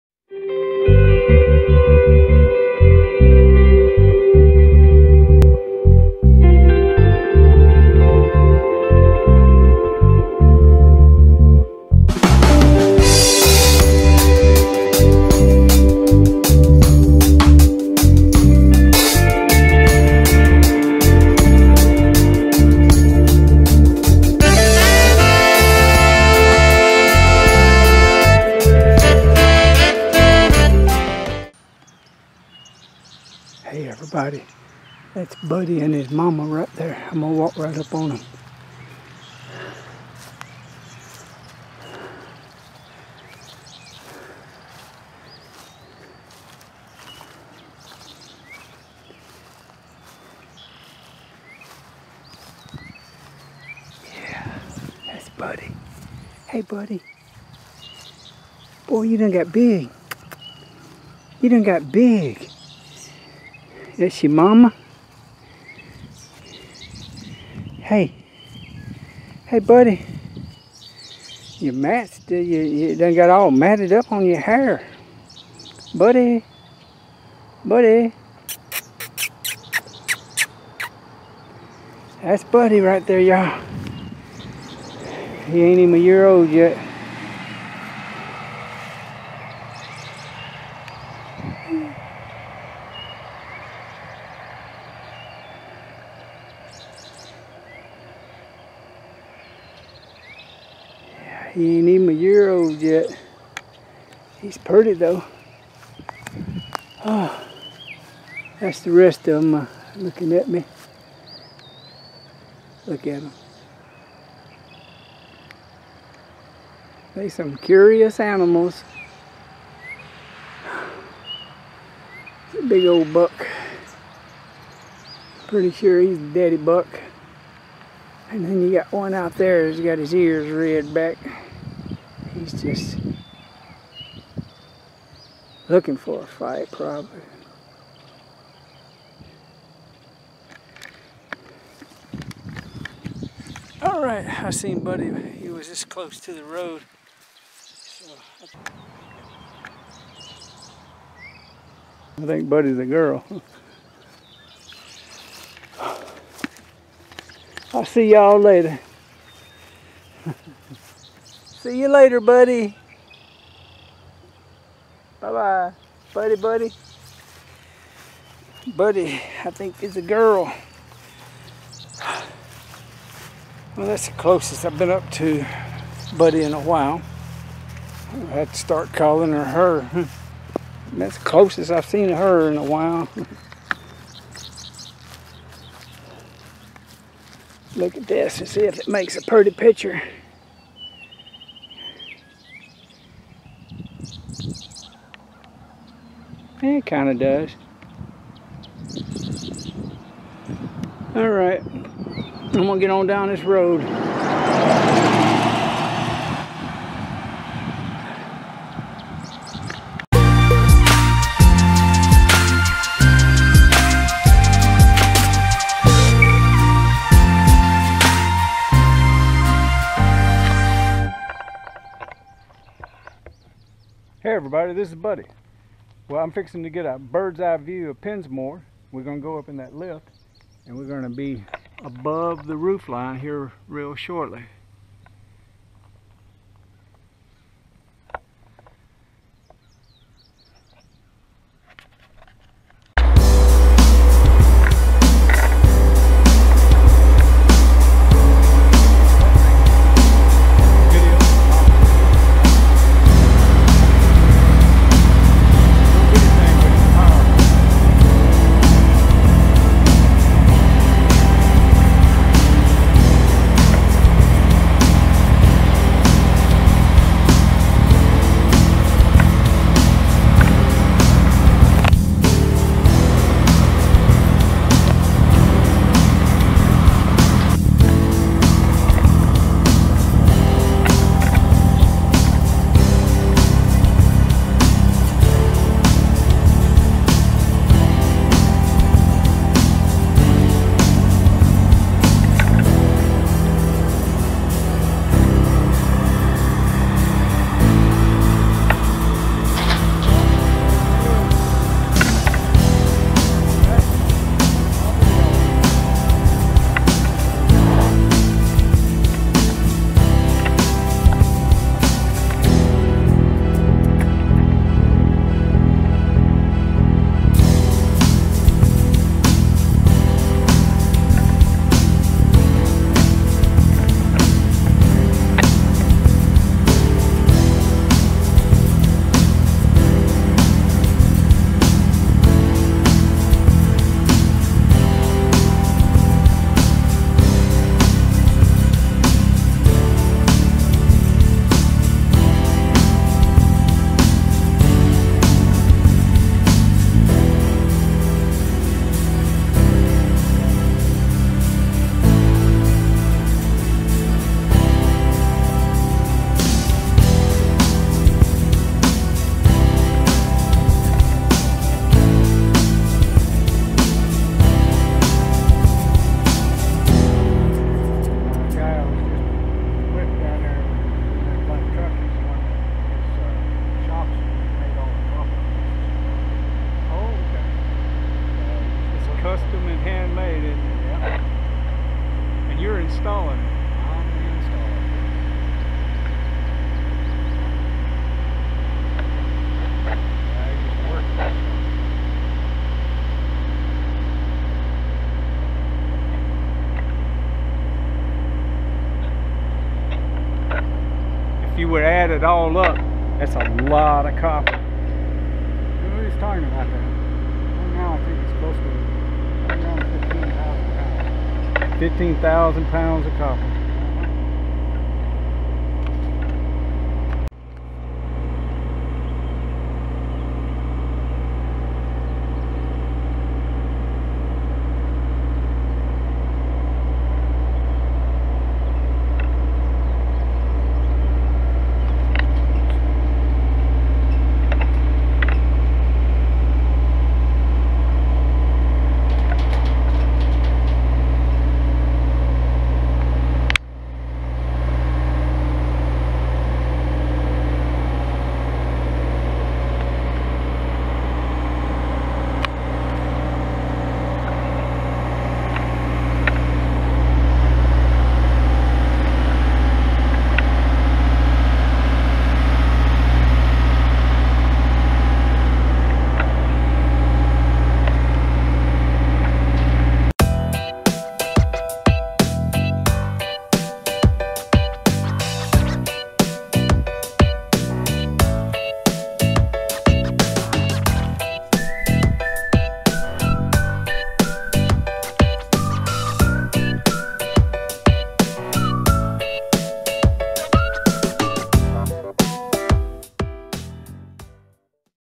Bike Ride video and never before seen clip of Pensmore Mansion
Pensmore Mansion is one of the largest homes in America, the castle-style Chateau Pensmore in southwest Missouri was designed to survive earthquakes, tornadoes, even a bomb blast. Owner Steven T. Huff told me in 2013 that the house should stand for 2,000 years.   I have lots of video of Pensmore that has never before seen.  I will post some of the 11 hours of video from time to time.

~ Buddha   : )